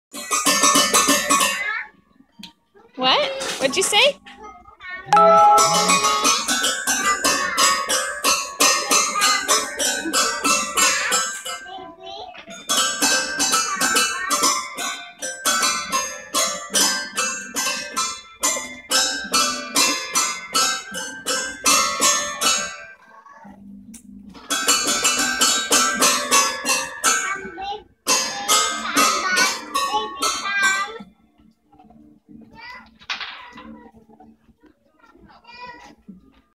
Baby come back: July 15 2014
The kid who was playing this piano before told her brother that his baby came back, so Adam thought that's what you said when you played the piano.